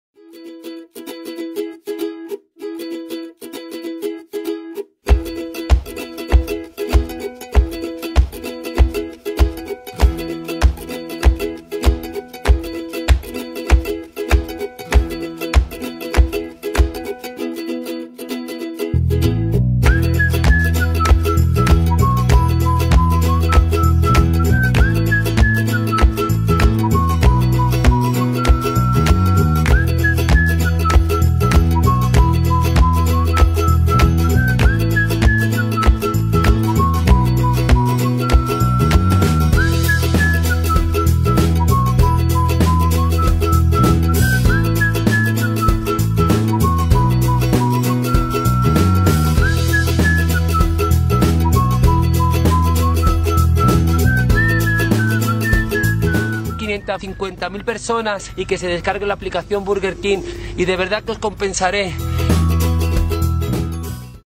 Burger King [iPhone] Video review by Stelapps
Bon app-etit!

Cuando salgo con mis amigas a comer no lo hago para gastar, sino por la compañía. Por ello, solíamos montar un buen follón al llegar al Burger: rebuscando entre cupones caducados, escudriñando enormes folletos de descuentos que a nadie le interesaban y hasta suplicando al dependiente por una rebajita, como si fuese un rastro... Por suerte eso queda lejos, ¡con la app oficial de Burger King tengo todas las promociones y descuentos exclusivos a un clic!

Ventajas

¡Si nos la bajamos hoy 550.000 personas Burger King nos dará una recompensa sorpresa!
Localiza al instante el Burger King más cercano.
Consulta la carta entera y descubre algo bueno para cada ocasión.

Desventajas

Parecía más rica cuando mi cartera rebosaba gracias a los cupones de papel :(